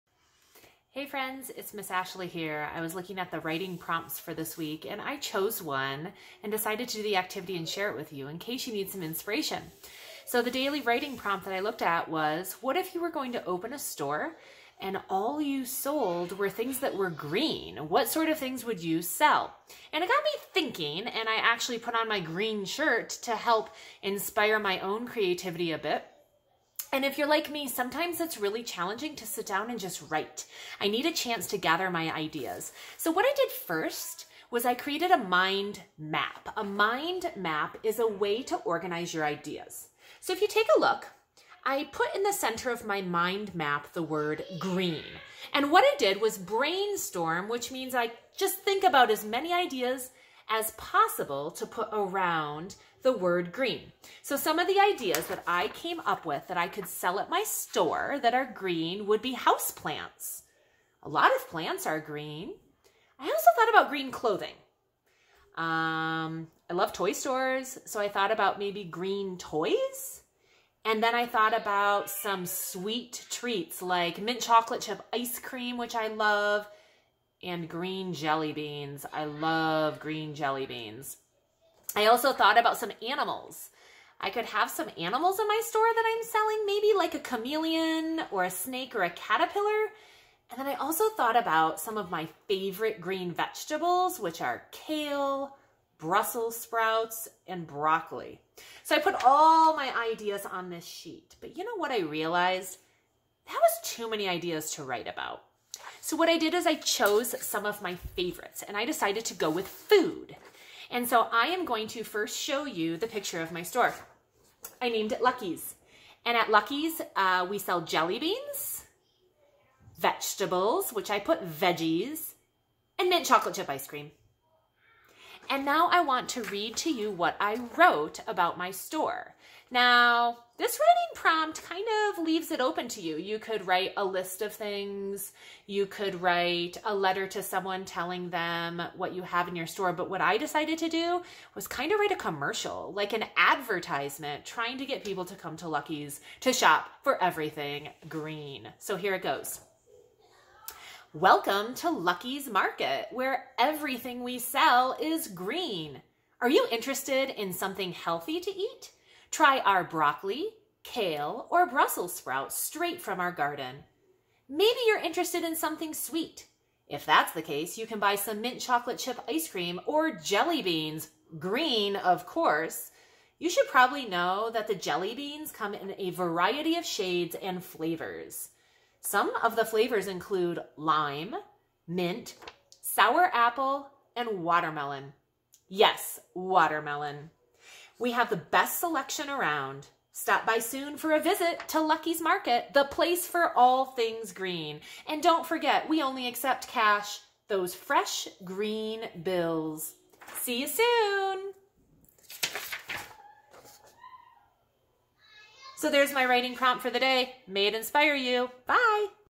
All Things Green
Ms. Ashley shares with you her writing prompt of "All Things Green".